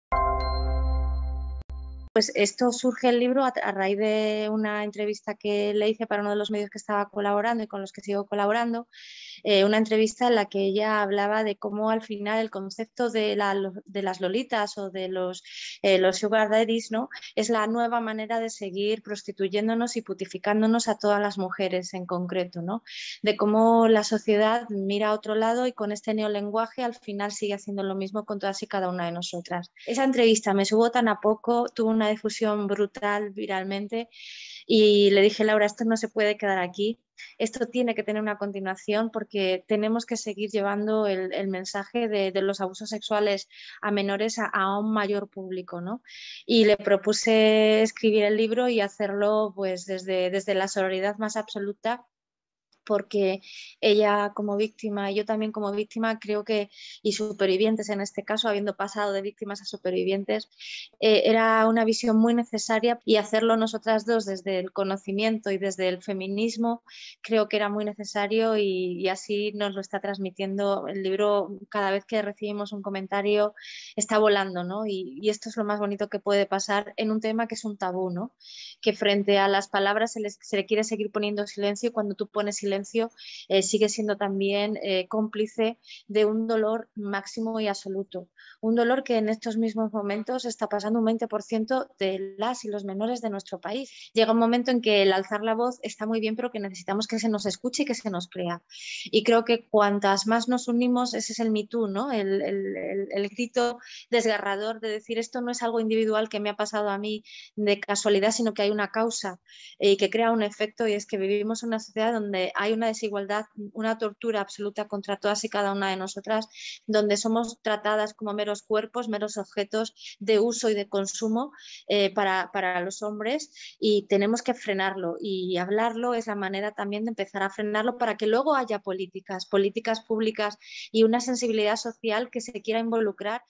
Nuria Coronado: "El concepto de 'sugar daddy' prostituye y putifica a las mujeres"
El silencio en torno al abuso no protege a las supervivientes de abuso, sino a sus agresores. Esa es la tesis de la que parten la periodista Nuria Coronado y la ilustradora Laura Strego en su último libro, "Lolita contra el lobo", el estremecedor relato de los abusos sexuales y malos tratos que Strego sufrió entre los 12 y los 17 años. Un ejemplar que nació a raíz de una entrevista entre las dos en la que, según recuerda Coronado en una charla con Efeminista, Strego narró "cómo el concepto de las Lolitas o de los 'sugar daddies' nos sigue prostituyendo y putificando a todas las mujeres, de cómo la sociedad mira hacia otro lado".